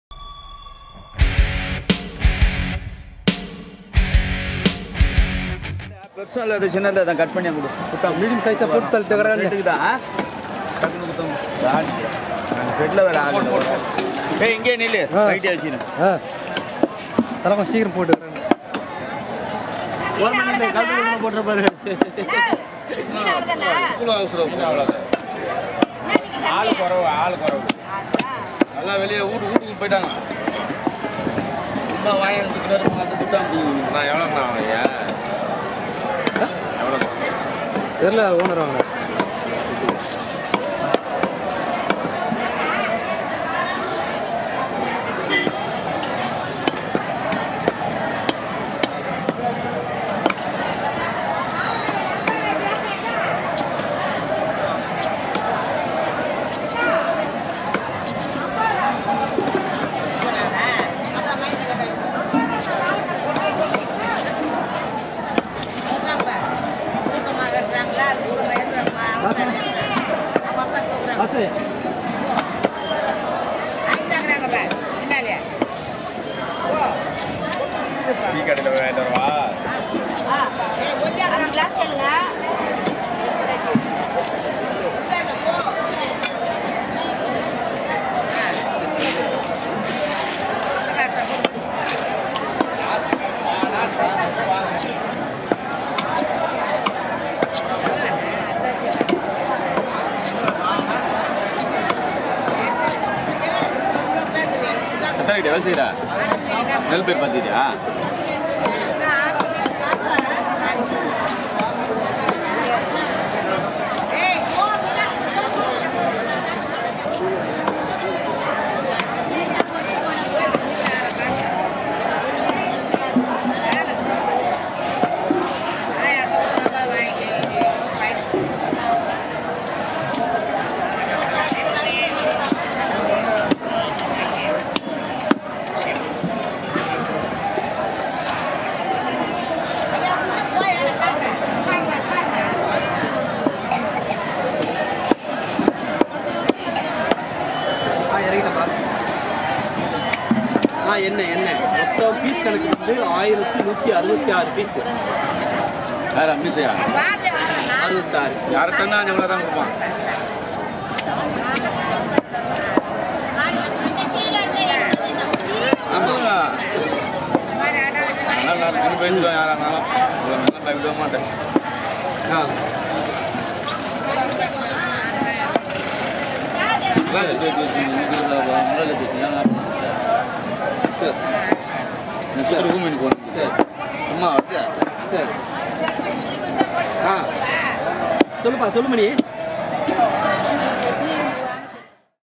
650 Rs /$8 Per Kg, Big Vanjaram Fish Cutting in Indian Fish Market
Here we have Showcased Seer Fish/ Vanjaram  Slicing and Cutting in Pazhaverkadu Fish Market, Chennai.